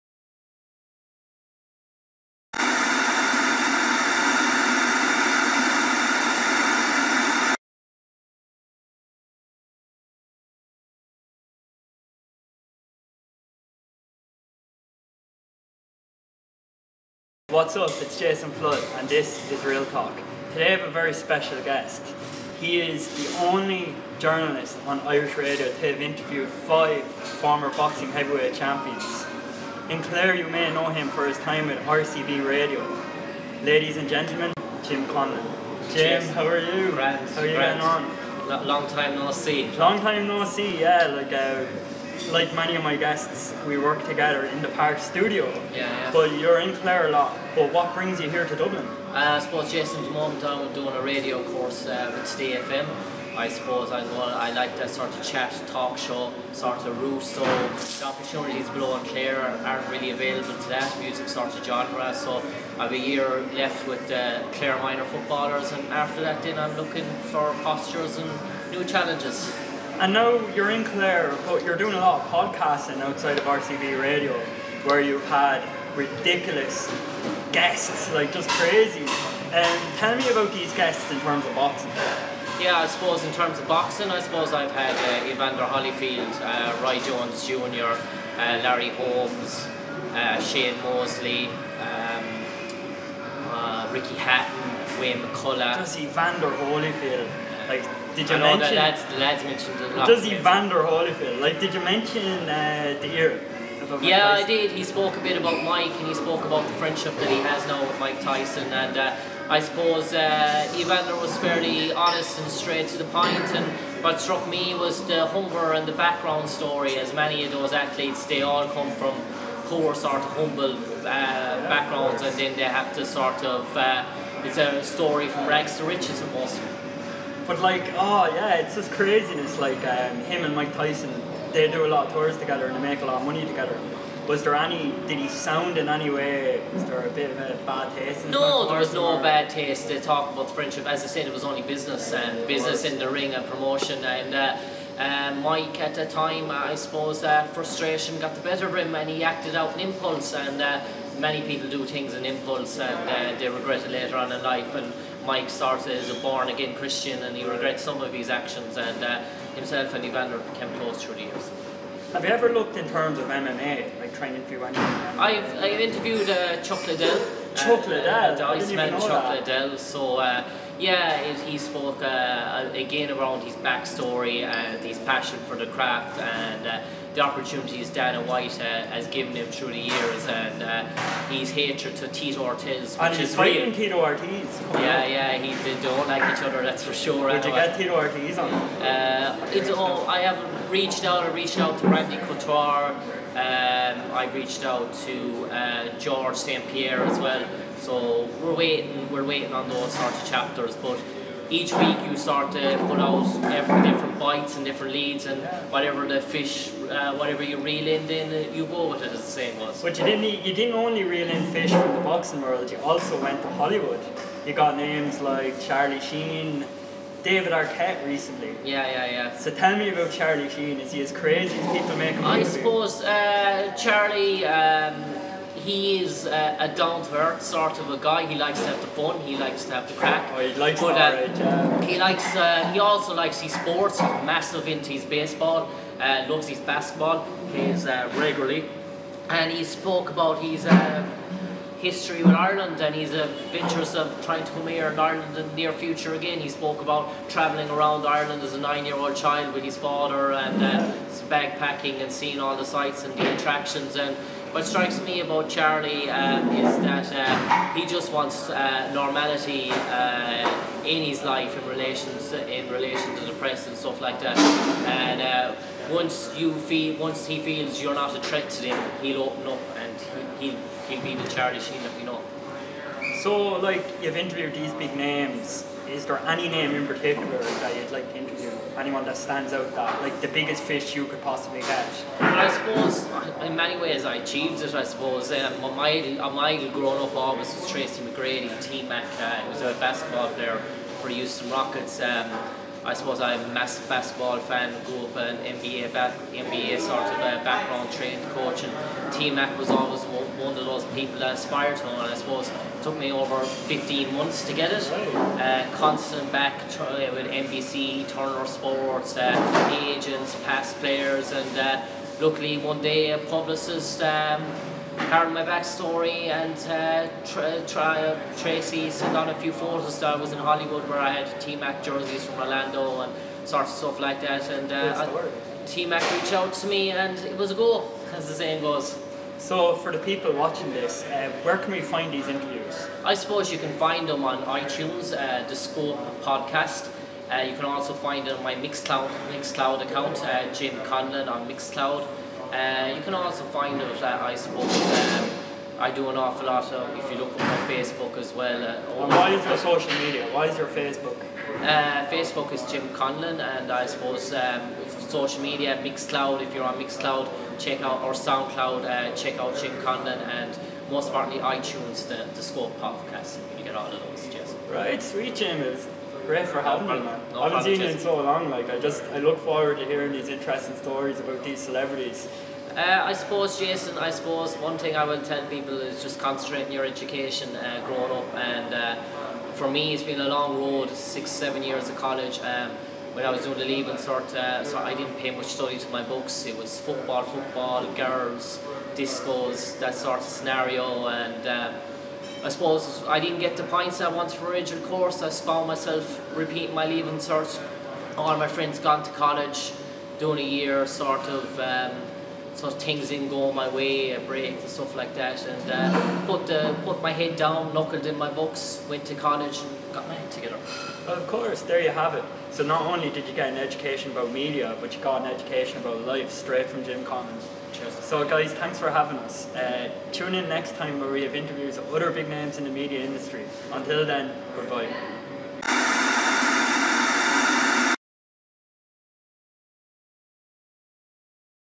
Interview with Jim Conlan | REALtalk
Jason Flood speaks to Jim Conlan. A radio presenter from Claire who is the only sports journalist on Irish air to have interviewed five former boxing heavyweight champions including names like Evander Holyfield. Jim has also interviewed Hollywood icons like Charlie Sheen and David Arquette. Jim speaks about all these names and more on REALtalk.